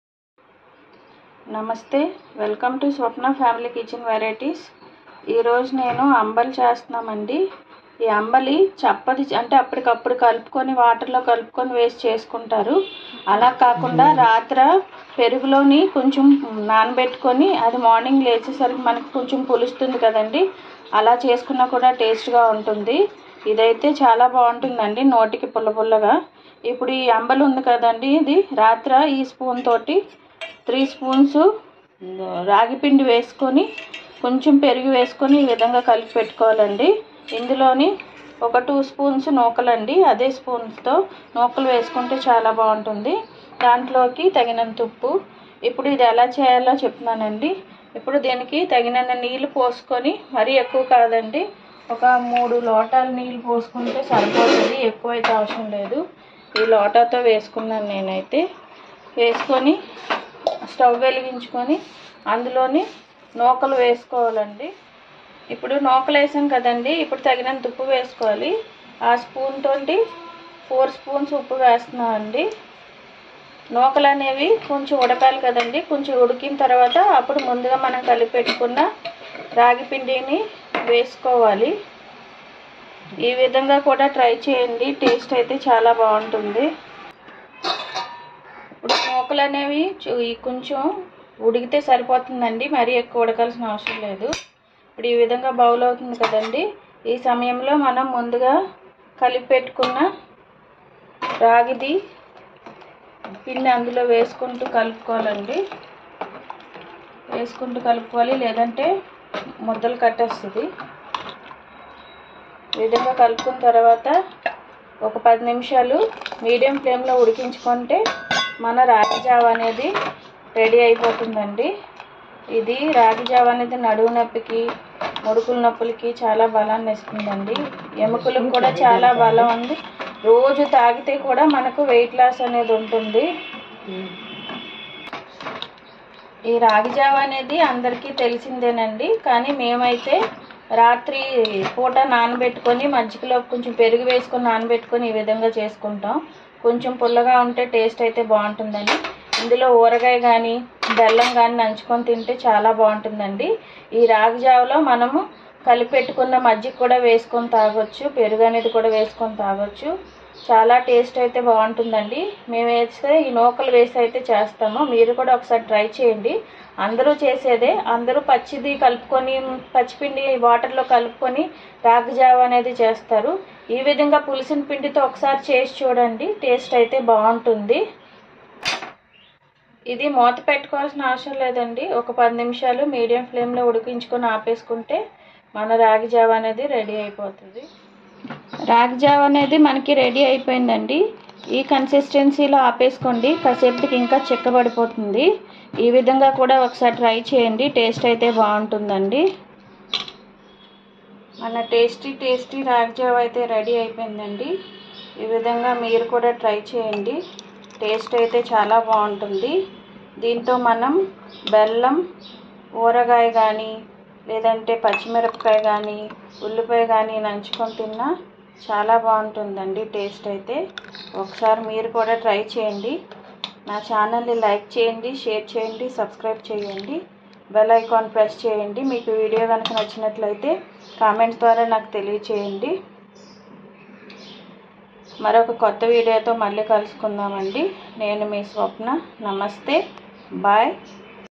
ఆరోగ్యంతో పాటు అందాన్ని మెరుగు పరిచే అమ్మమ్మల కాలం నాటి రాగి జావా కొత్తగా పెరుగు తో ఇలా చేయండి 👌👌😋😋
This ragi malt is one of the south indian breakfast recipe in early morning sesstion//it a healthy /nutritious drink/made with curd

నేను చేసిన రాగి జావా పెరుగు లో పిండిని కలిపి ఒక రాత్రి ముందు నాన్న పెట్టుకుని మార్నింగ్ వీడియో లో చూపించిన విదంగా చేయండి కొంచెమంత నూకలు గాని లేదా రైస్ రవ్వ గాని వేసి ఇలా చేయండి రుచి చాలా బాగుంటుంది 😋😋😋😋😋

రాగి జావా మనకు ఎంతో ఆరోగ్యాకరమైన చిరుధాన్యాలలో ఒకటి ఈ రాగులు ఎముకులకు ఎంతో బలాన్ని ఇచ్చే ఈ రాగి జావా ని రోజు తాగండి

My dear family members